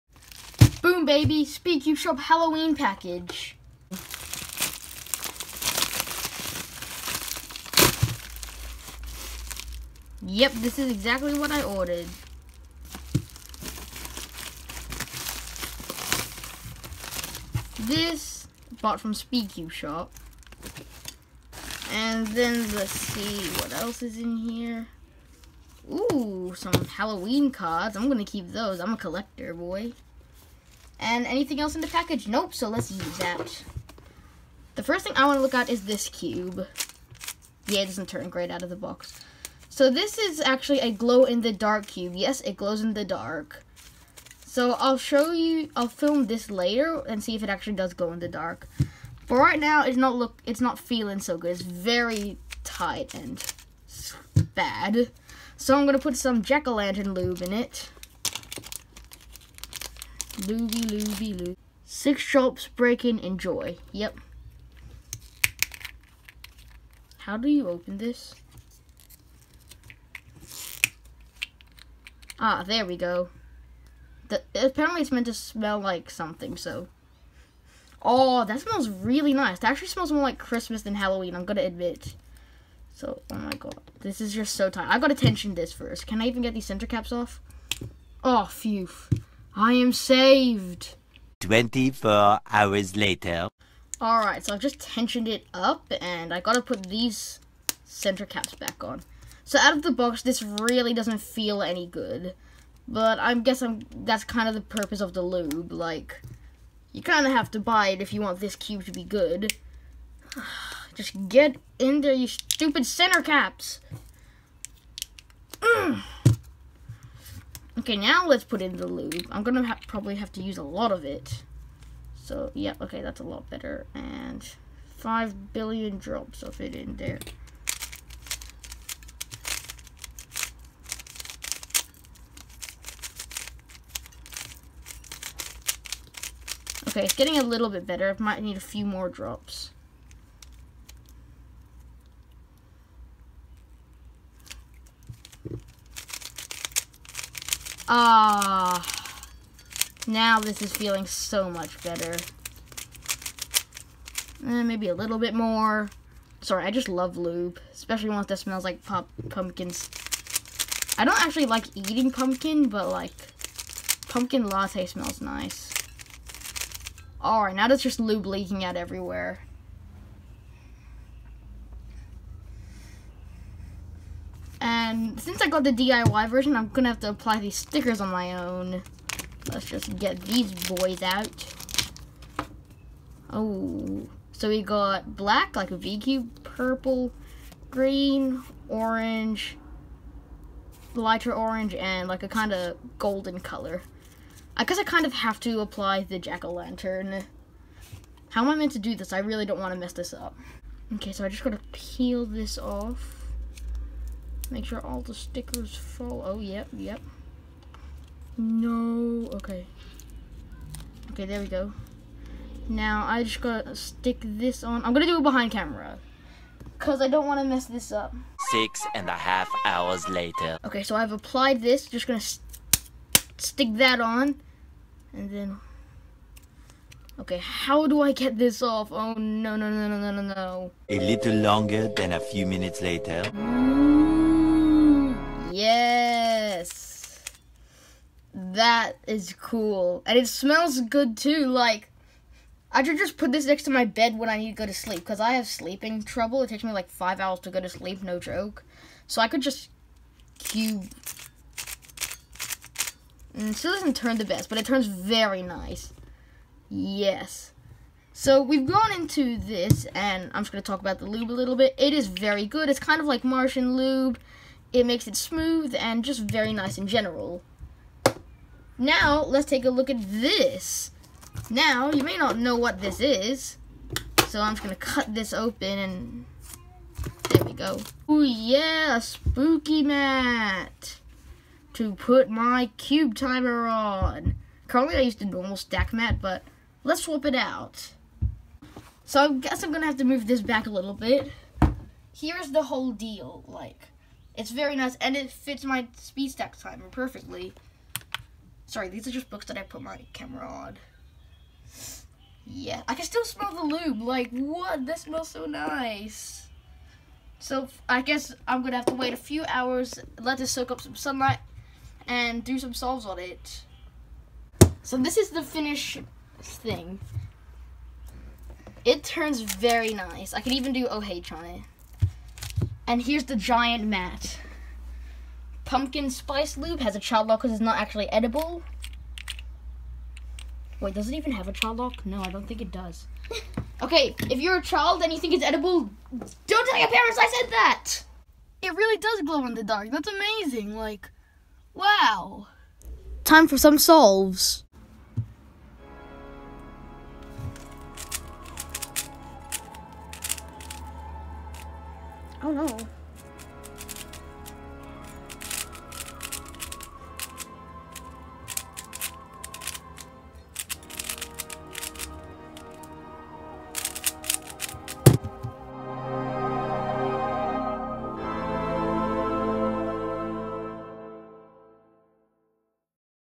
SPOOKY Hallowwen cube unboxing! (Jack-o-lantern lube and cube, halloween mat)
The cubes where bought from SpeedCubeShop, a great store.
Turning: 6/10
Awesomeness: 8/10
Everything else: 8.5/10